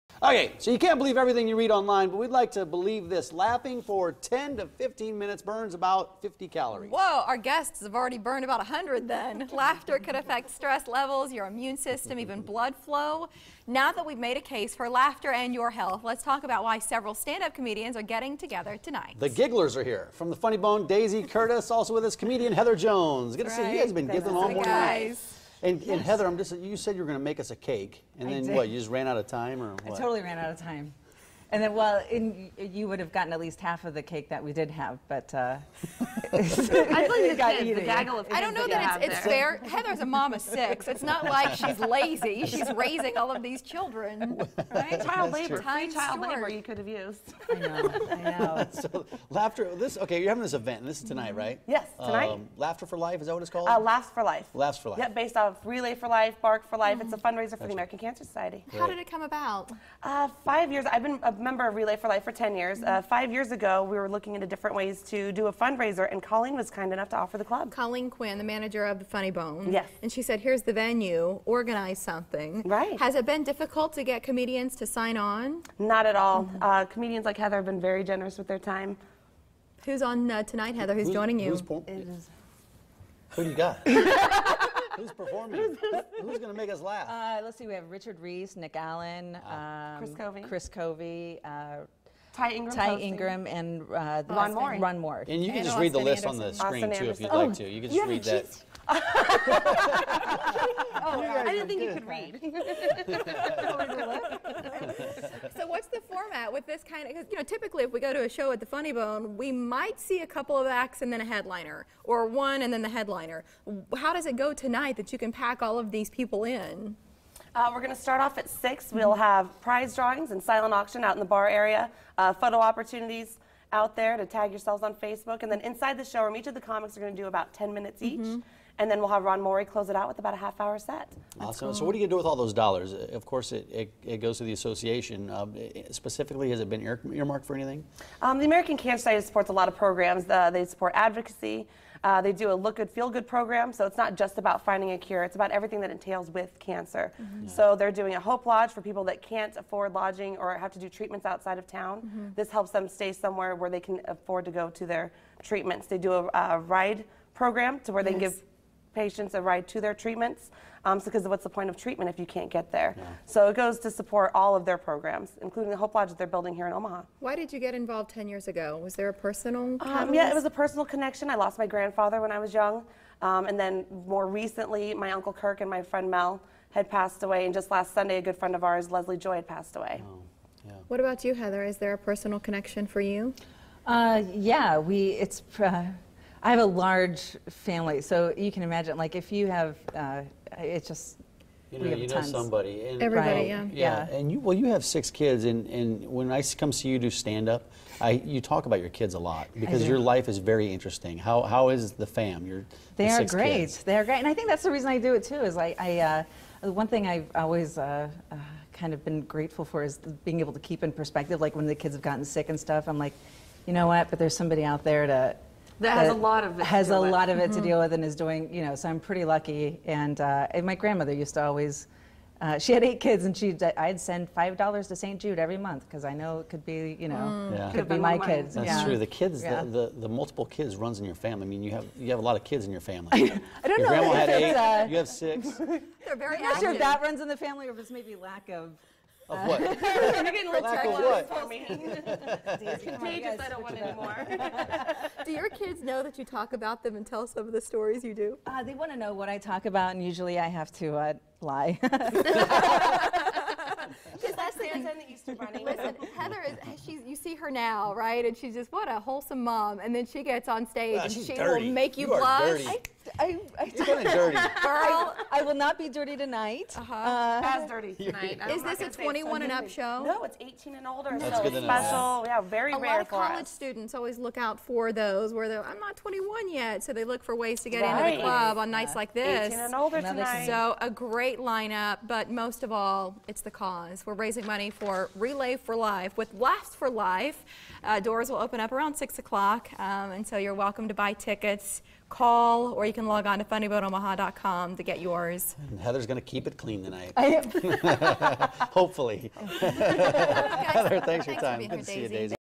Laughs for Life
Laughter is always the best medicine, that's why the Funny Bone Omaha will host the 5th Annual Laughs for Life!  Net proceeds will go to benefit the American Cancer Society.  Join us to find out who will be there and what you can win!